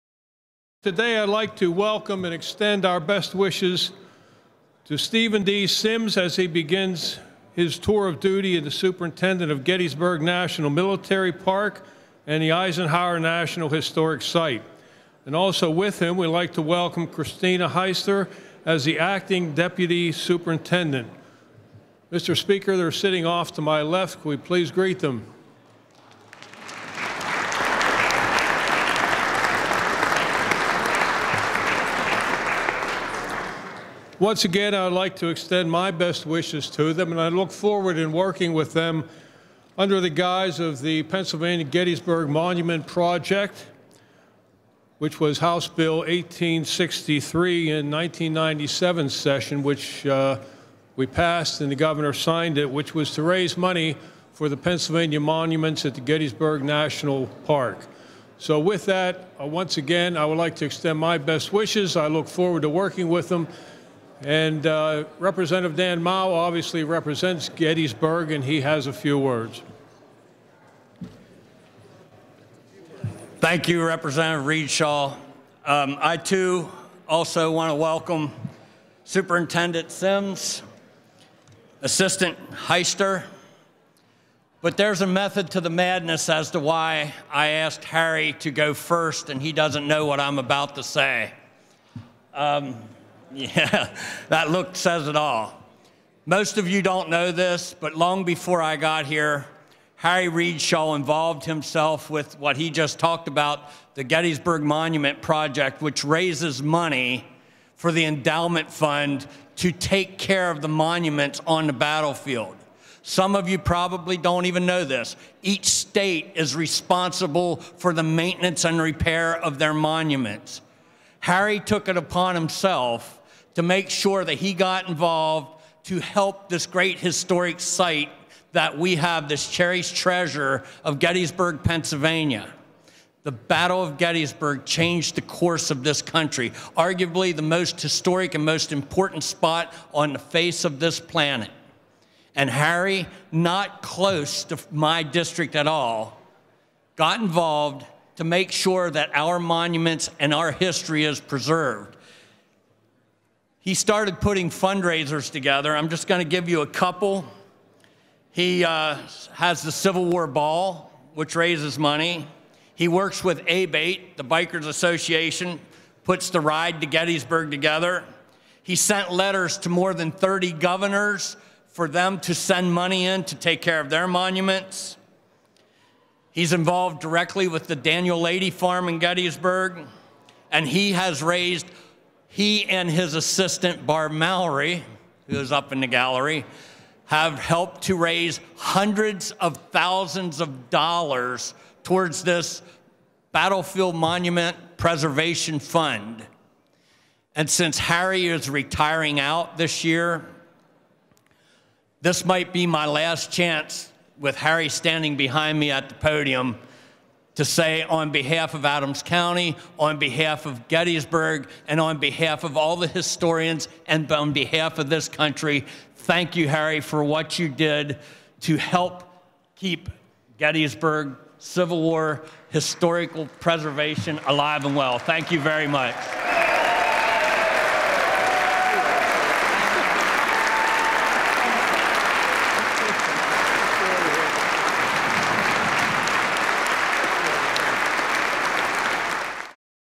The Gettysburg Monument Project
State Rep. Dan Moul recognizes those who have made efforts to preserve the monuments at the Gettysburg Battle Field.